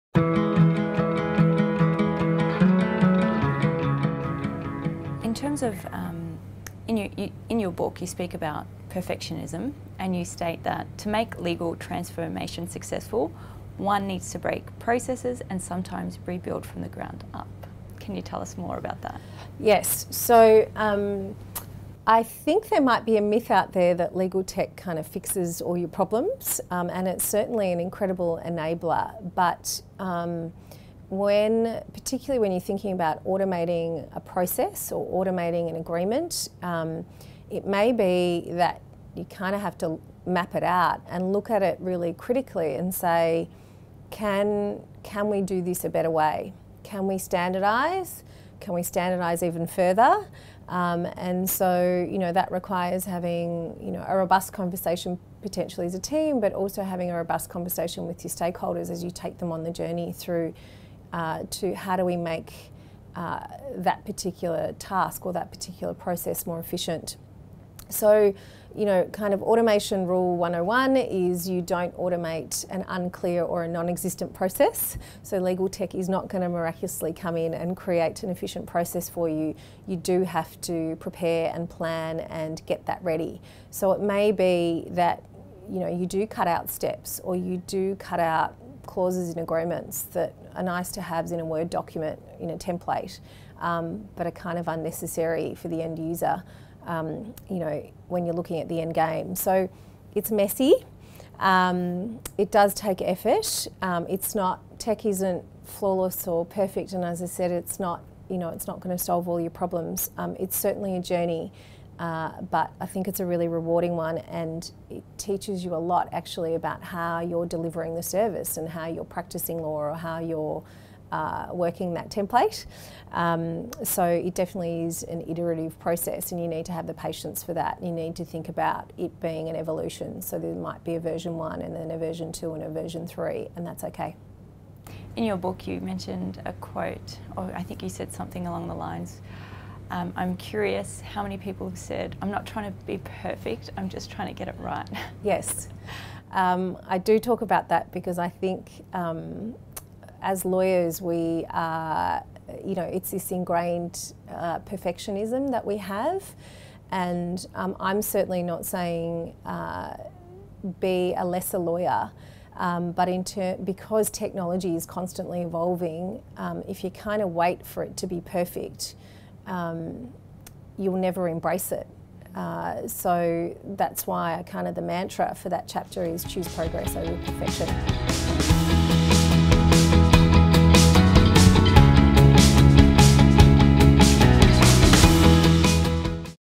Anna Lozynski | Choose Progress not perfection
Anna Lozynski is General Counsel at L’Oreal, a thought leader and author of the book titled Legally Innovative: How to Maximise Your Legal W.O.W. (Ways of Working).

In this interview with our host, Louise Hvala, Anna describes how legal technology isn’t going to solve all of a legal team’s problems, what other issues need to be resolved and why progress needs to take priority over perfection.

Changing lives. Helping people with their legal careers. Building high performing legal teams.